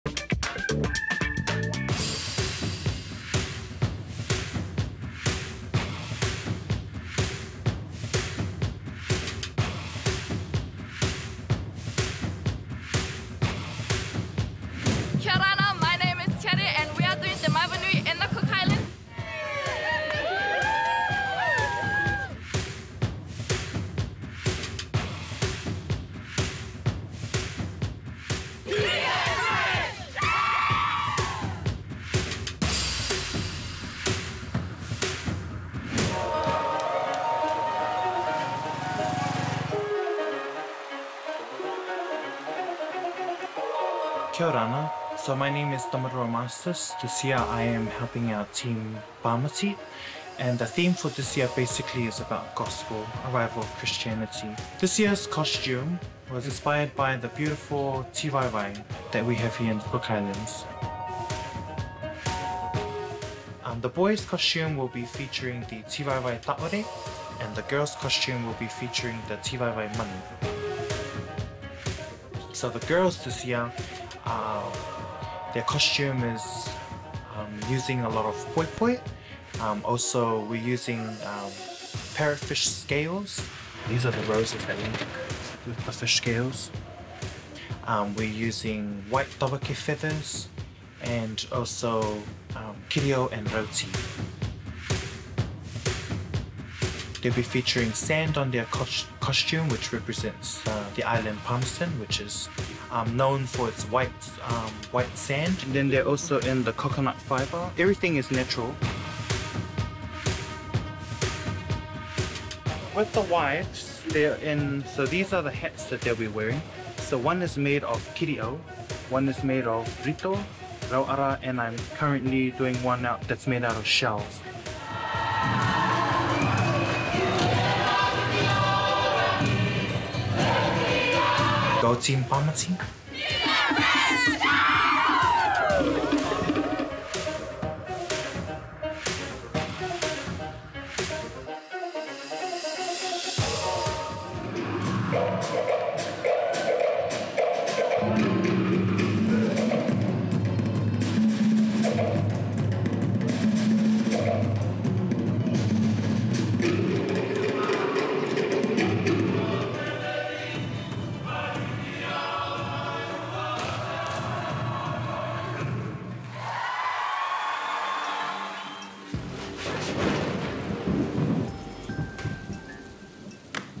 Making Costumes for Te Maeva Nui | Keepin It Fresh
Welcome to the beautiful Cook Islands! The land of beautiful smiles and vibrant traditions.

In this segment, we go into the Marsters house to take a tour behind the scenes of Te Maeva Nui costume making. We get an up-close look into what it takes to make garments fit for a stage and jaw-dropping performance. From parrotfish scales to sand... a lot of intricate details go into making a Kuki fit.

Keepin It Fresh brings you all the freshness happening in our Moana communities, from Siva Afi Festivals to Pacific Cabarets.